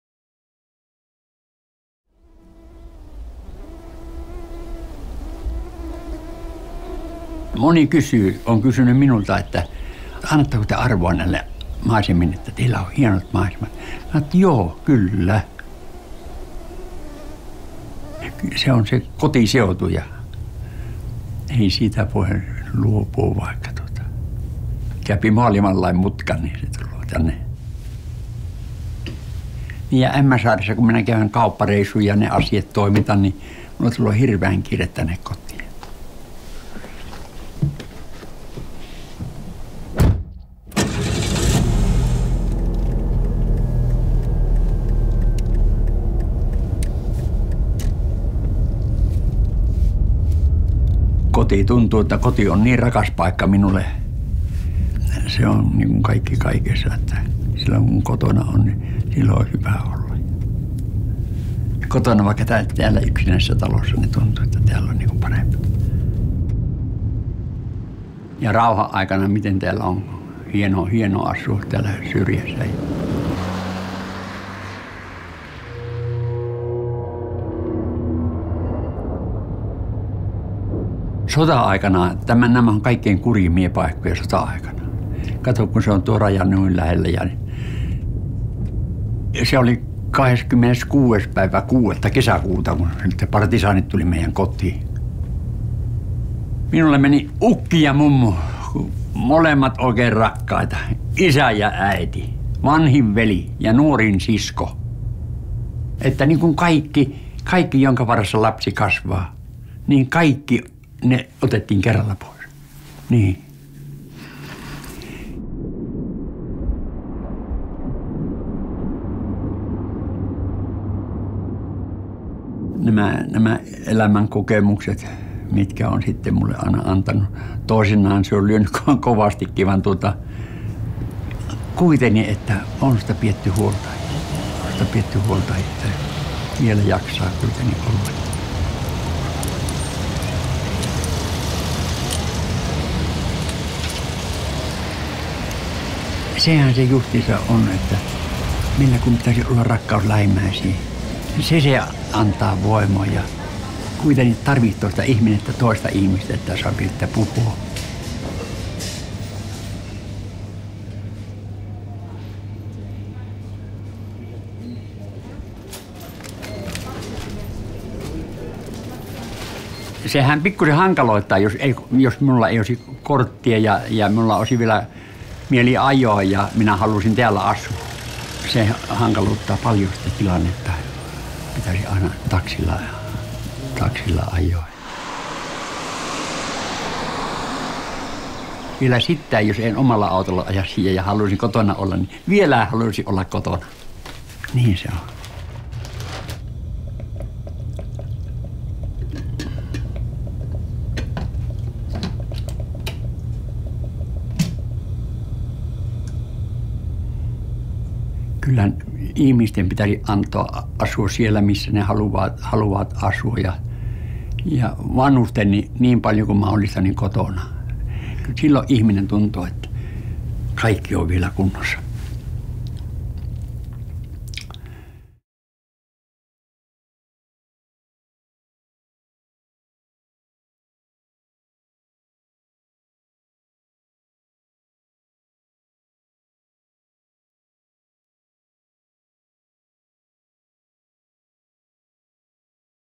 Väinö, 81 - Koti on ihmisen paras paikka
Vaikka lähikauppaan on 60 kilometrin matka, Väinön hartain toive on saada asua omassa kodissaan.

Entä, jos ajokortti otettaisiin pois?